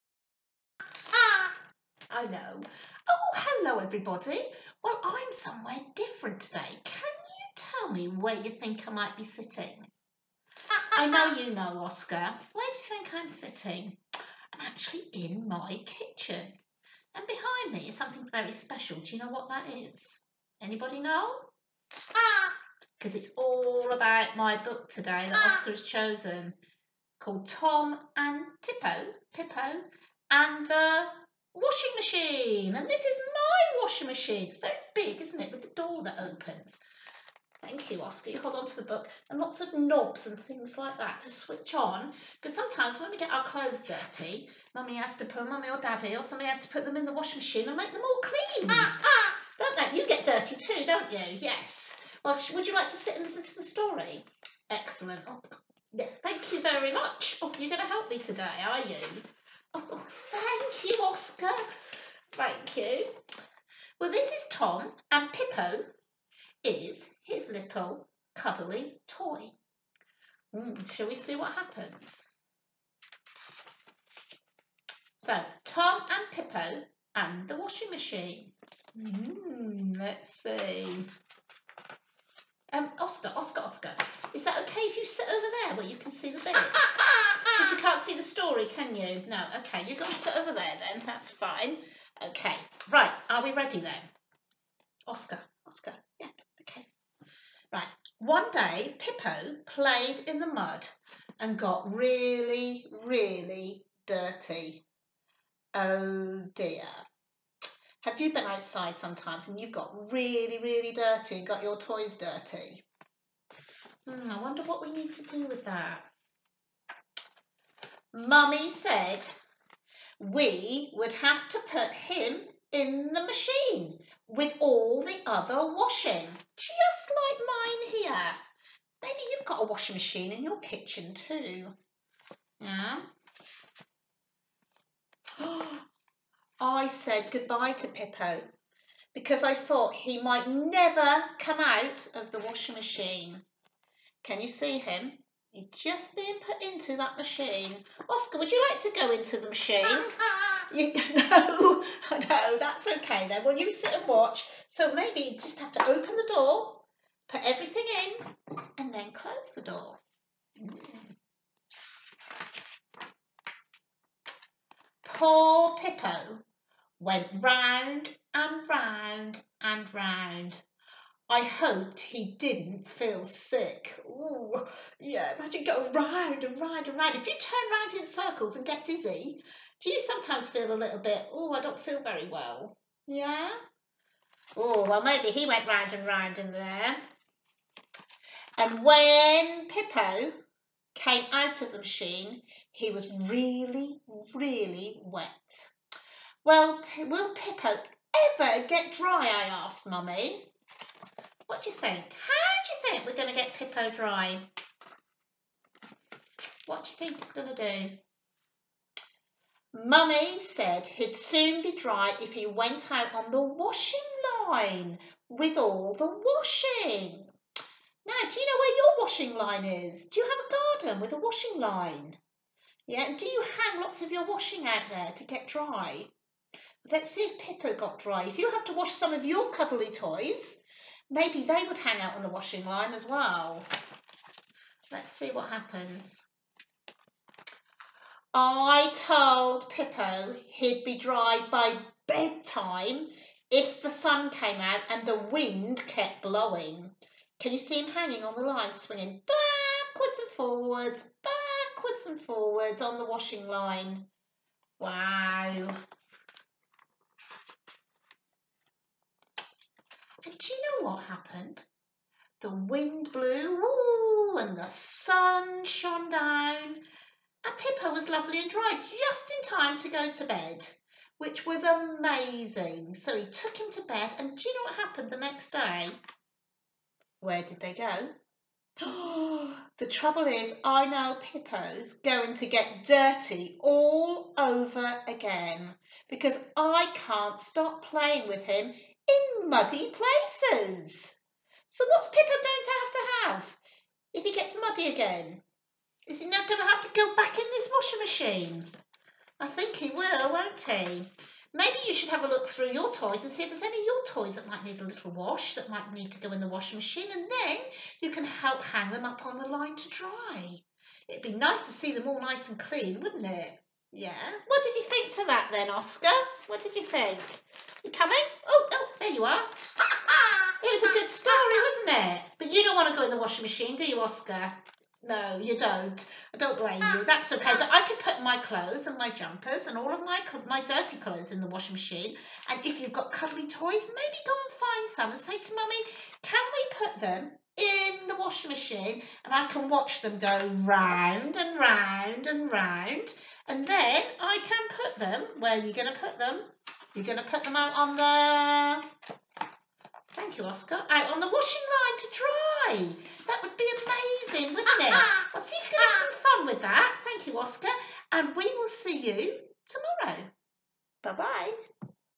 Storytime with Helen... Tom and Pippo and The Washing Machine | Marcham Little Angels
Storytime for kids, sit down, relax and get cosy with our online fun storytime for children. Suitable for babies, toddlers, and preschoolers. Today's storytime for children is Time and Pippo and The Washing Machine.